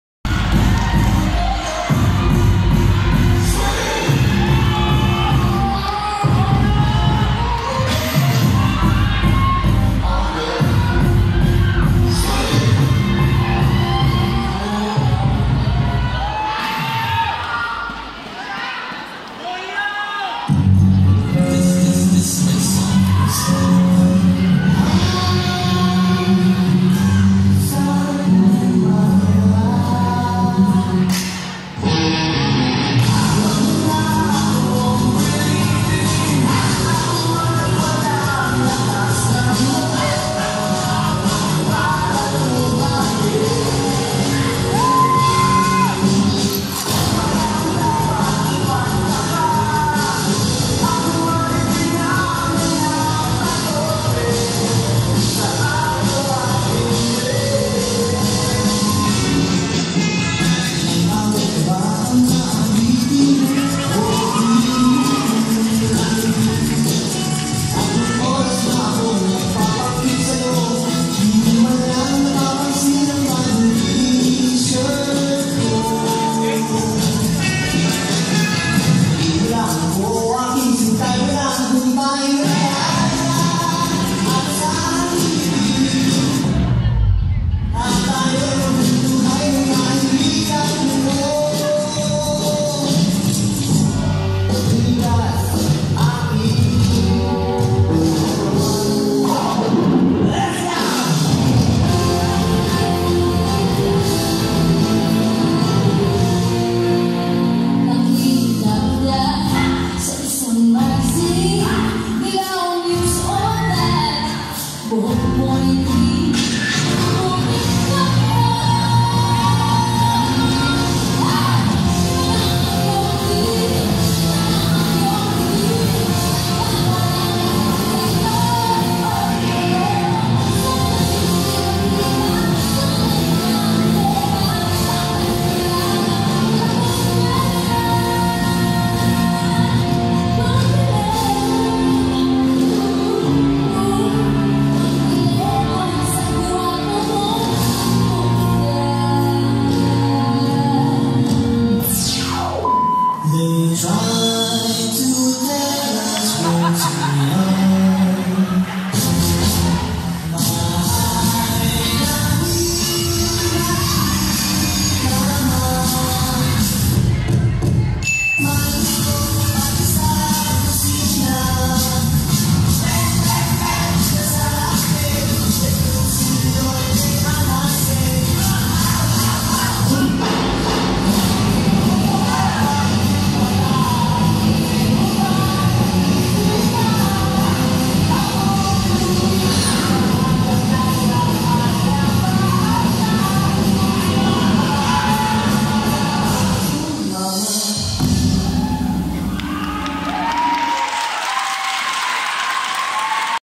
Protocol 2017 - Cognizant Hiyaw
This 2017 Protocol is held at Tanghalang Pasigueño, an annual hip hop dance competition amongst the BPO companies.